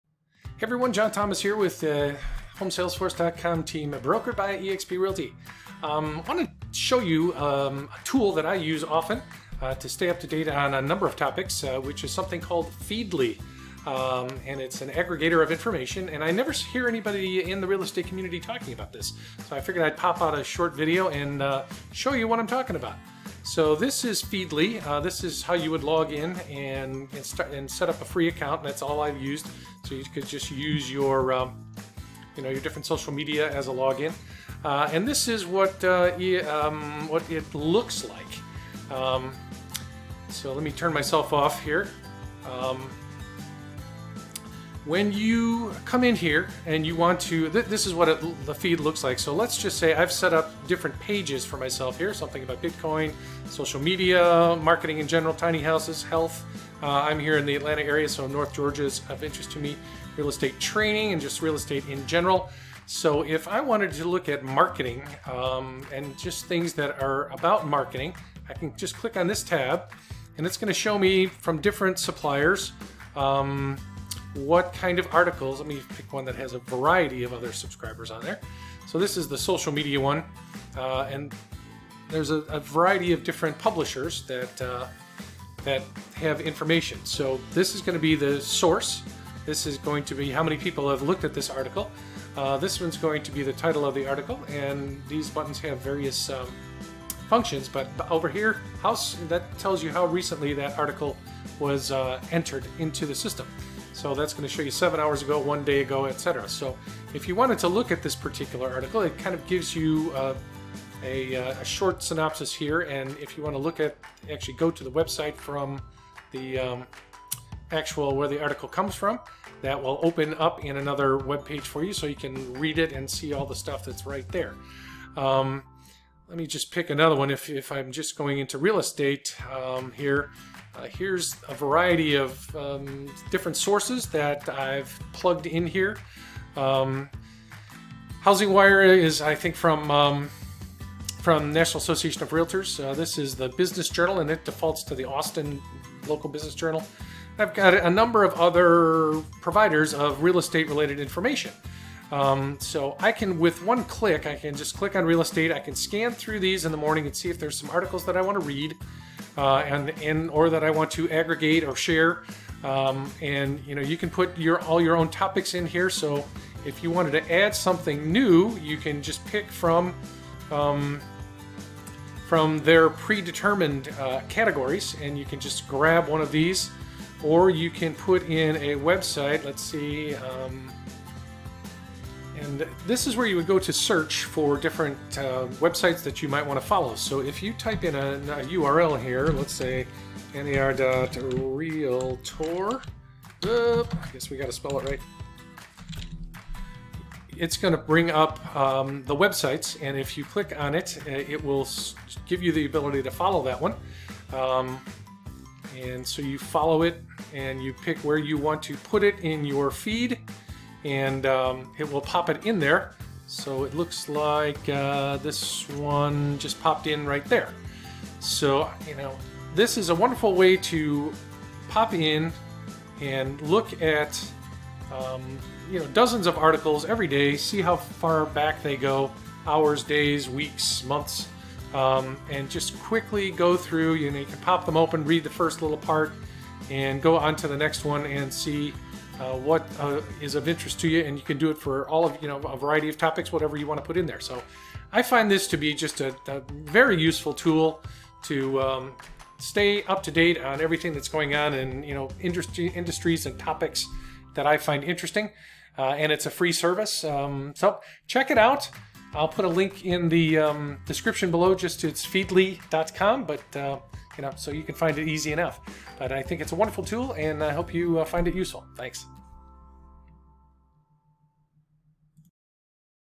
Feedly tutorial
Here is a tool I use all the time that I never hear anyone talking about in the real estate industry. It free and saves me tons of time in keeping up with my industry.